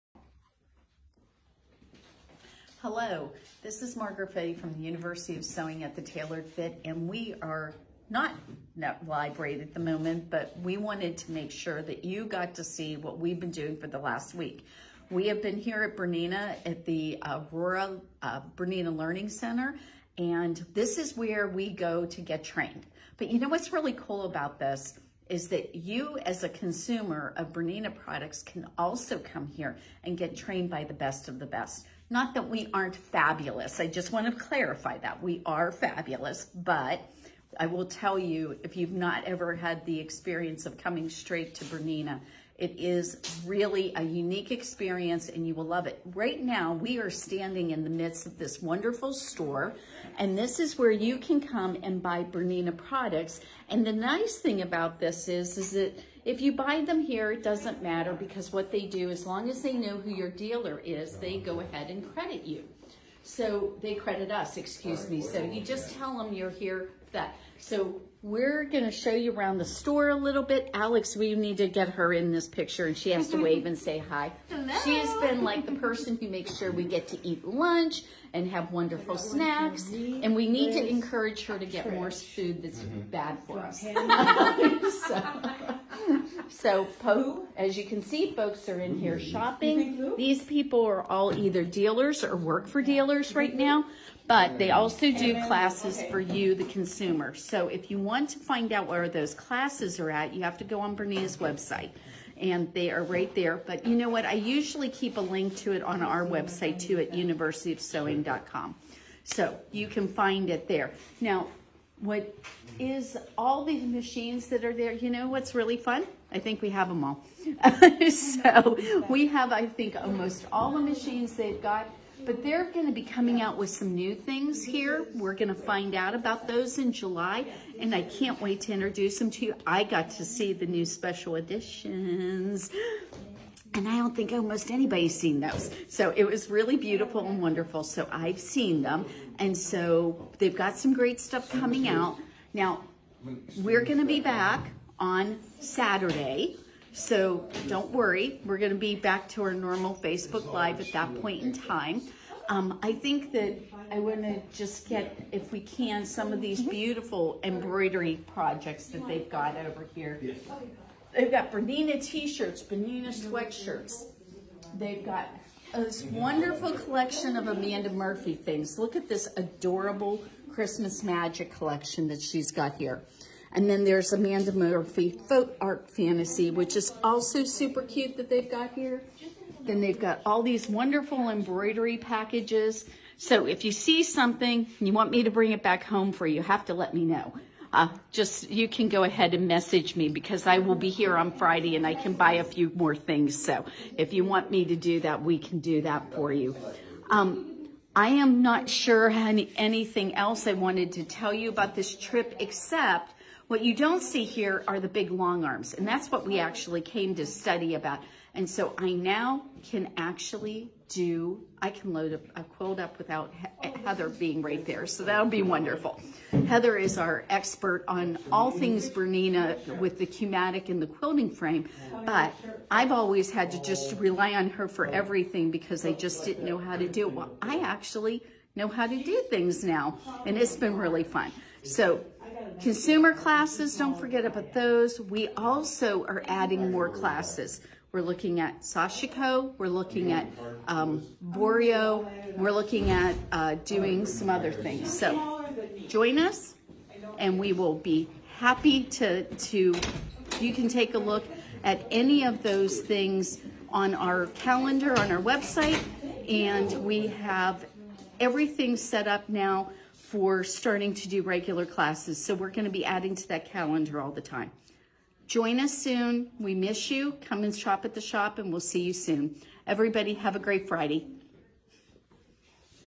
University of Sewing Facebook Live Friday 6/25/2021
Margaret went to Aurora IL to train on the LongArm BERNINA's.  She's in the BERNINA of America's gift shop for this Facebook Live.